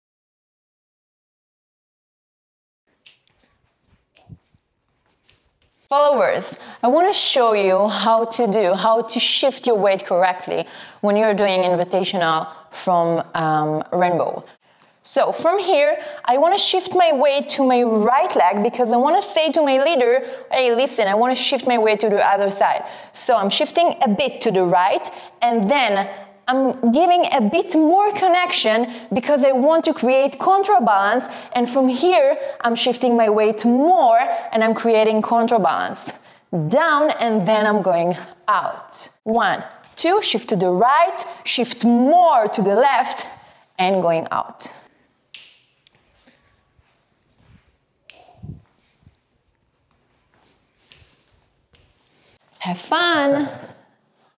SHIFT your WEIGHT in West Coast Swing
In this video we talk about shifting weight during an invitational. Now Karin shifts her weight as far out that she would fall if Rolf let go. We call that leverage - when you need your partner to support your not only your weight, but also your balance. Practice at your own risk :)